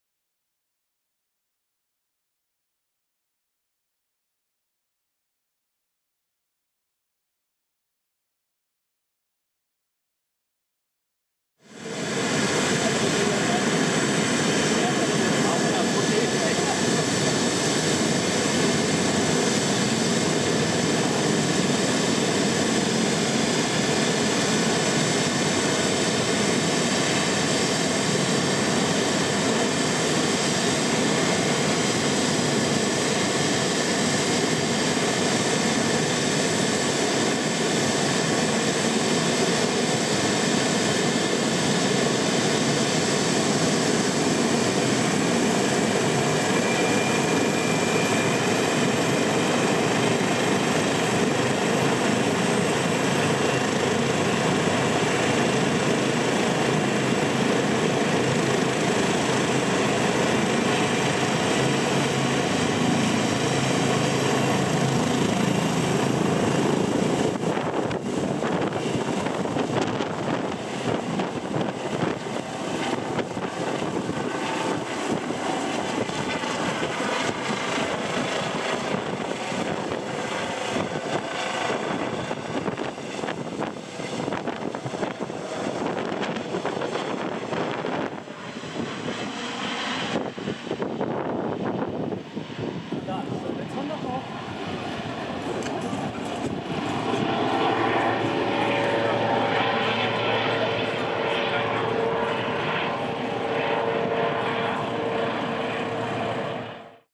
Start D-HDMR Christoph 27 Spardorf 28.05.2023
Start unmittelbar neben Kindergarten Spatzennest nach Notarzteinsatz in Spardorf